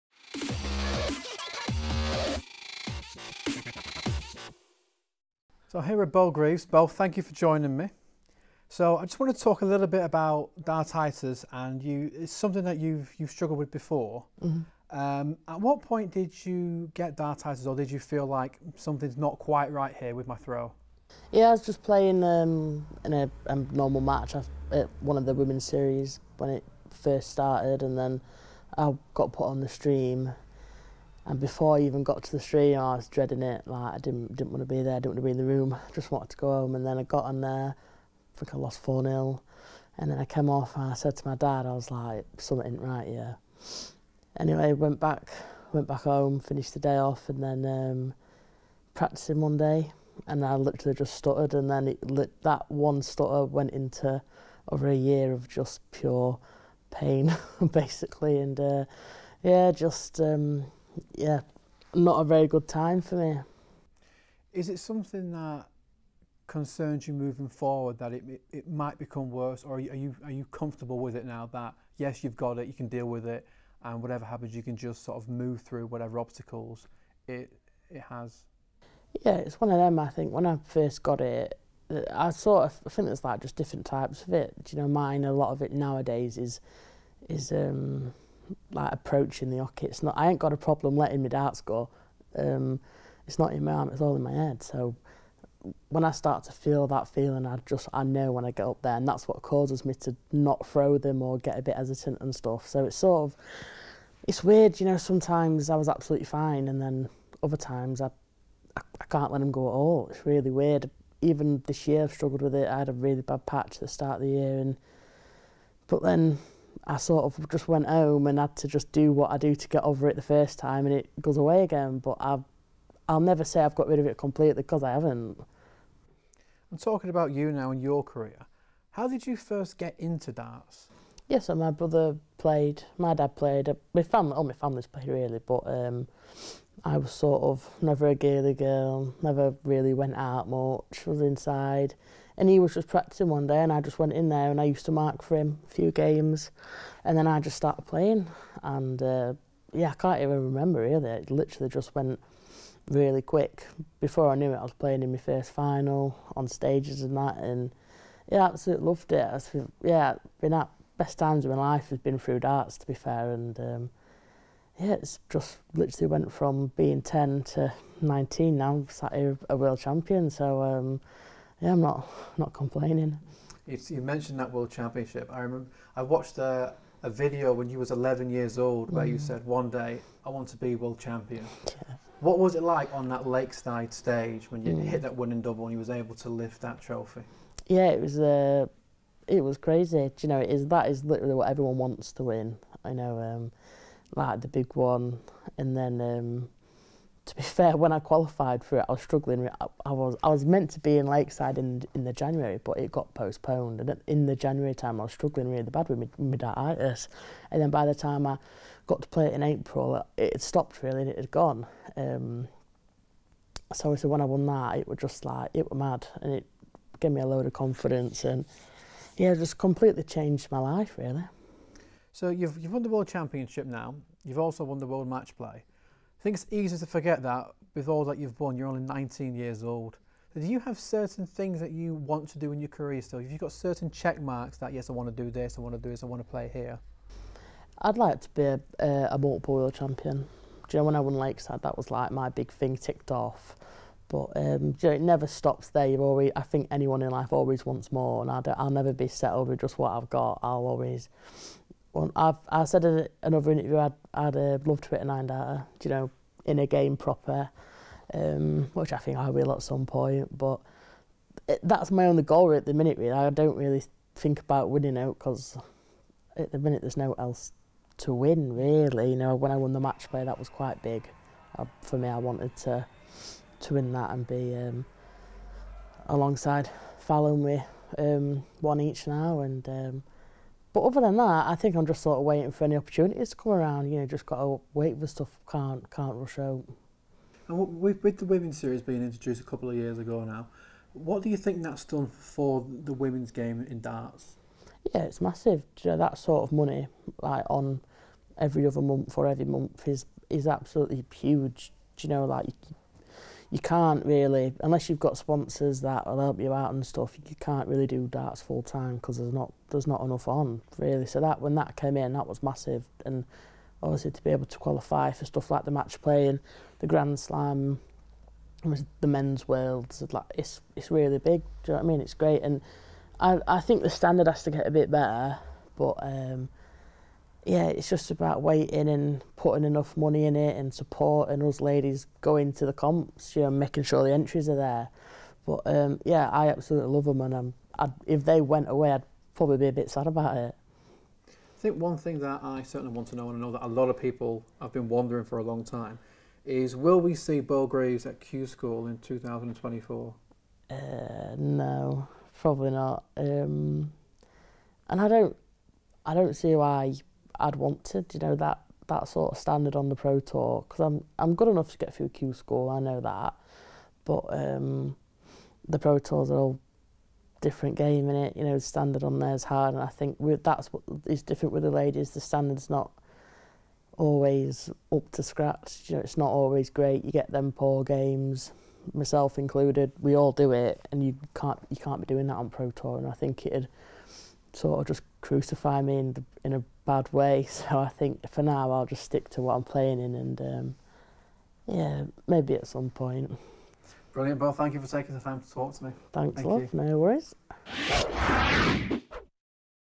Beau Greaves talks dartitis, Q-School and World Championship aspirations
World Matchplay and World Champion Beau Greaves sat down with Connor Cain to talk about her struggles with dartitis, 2024 Q-School, her dreams of another World Championship and more